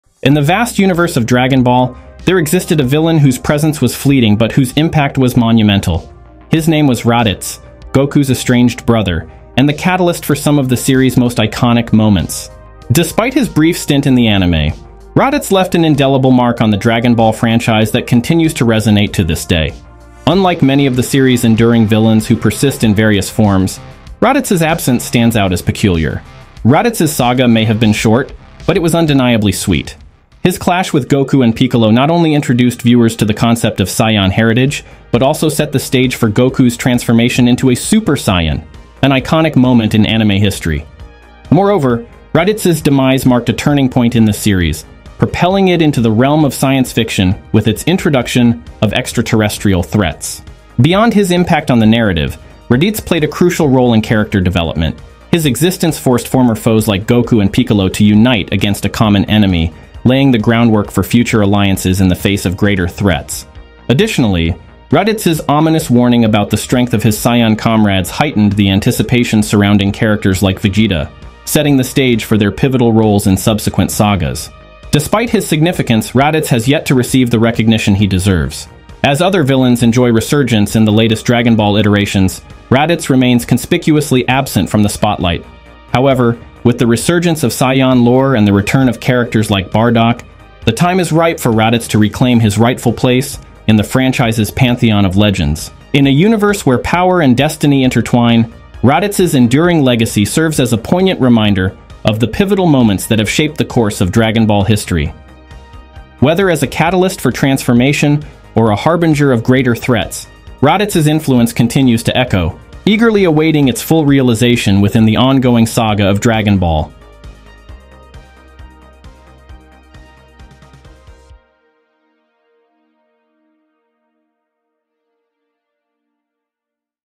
DRAGON BALL: Raditz's Impact on Dragon Ball History
Let's explore how Raditz's brief yet pivotal appearance shaped the course of Dragon Ball history, serving as both a catalyst for transformative moments and a harbinger of greater threats to come.

Join us as we unravel the layers of Raditz's influence, delving into his impact on character development, narrative progression, and the overarching themes of power and destiny within the Dragon Ball universe. From sparking Goku's iconic Super Saiyan transformation to foreshadowing looming dangers, Raditz's legacy reverberates throughout the series, eagerly awaiting its full realization in the ongoing saga.